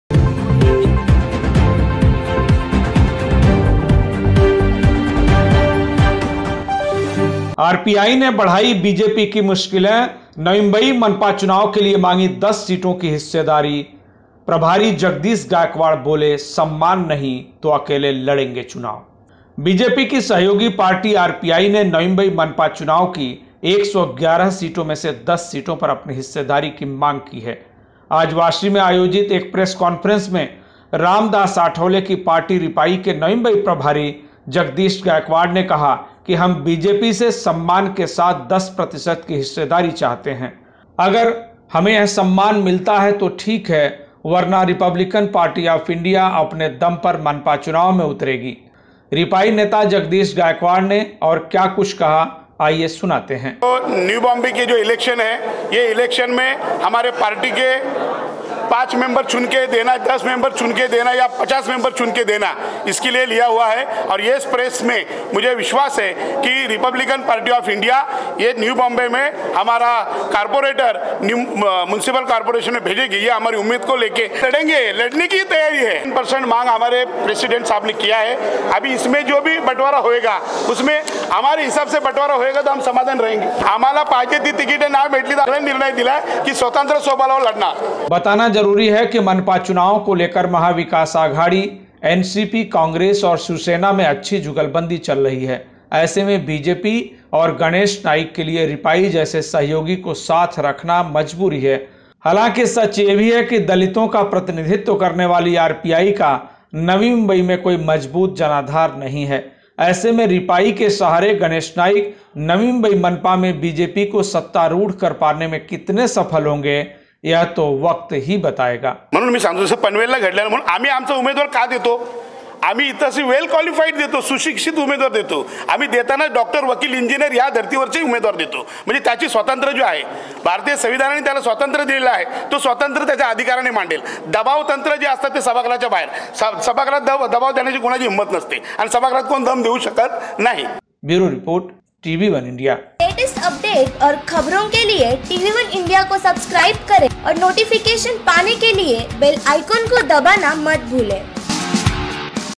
RPI अपने दम लड़ेगी नवी मुंबई NMMC चुनाव | Jagdish Gaikwad ने BJP से मांगी 10 सीटों की हिस्सेदारी
RPI अपने दम लड़ेगी नवी मुंबई NMMCElection2021 | JagdishGaikwad ने GaneshNaikBJP से मांगी 10 सीटों की हिस्सेदारी